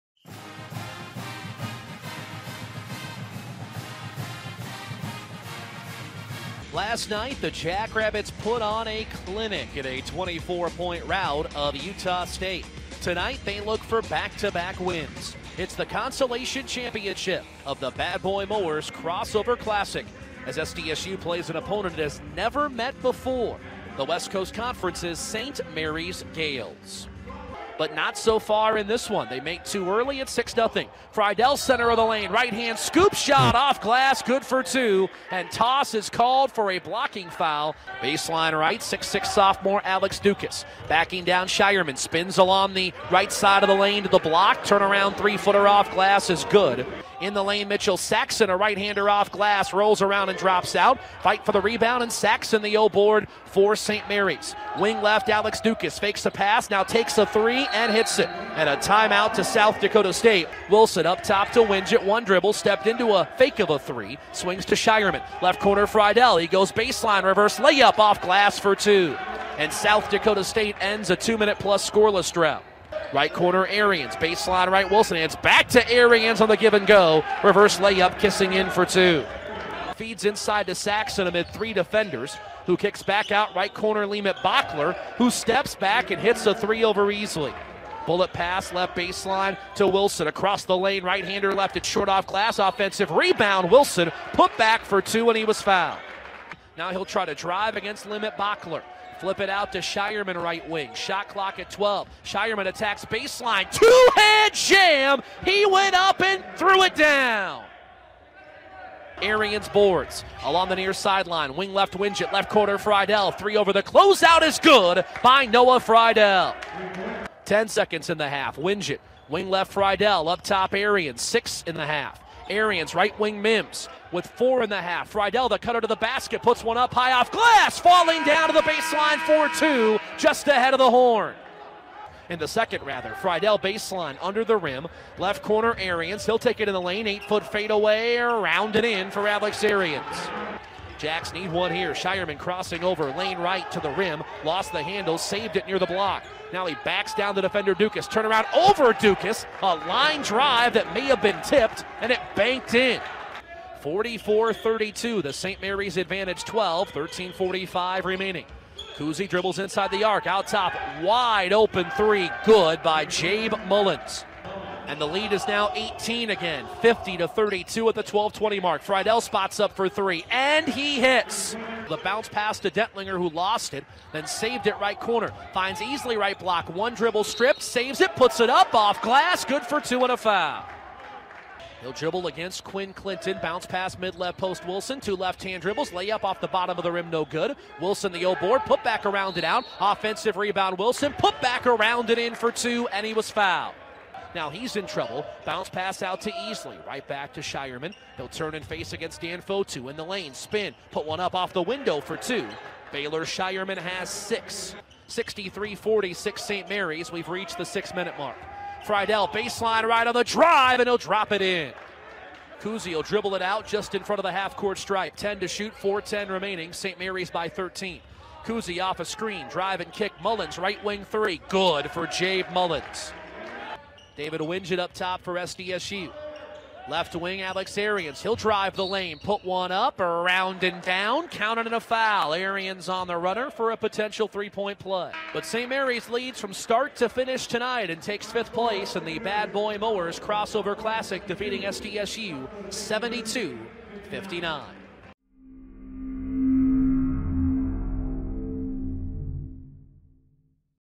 Men's Basketball Highlights vs Saint Mary's (11.27.2020)
SDSU drops its final game in the Bad Boy Mowers Crossover Classic to Utah State, 72-59.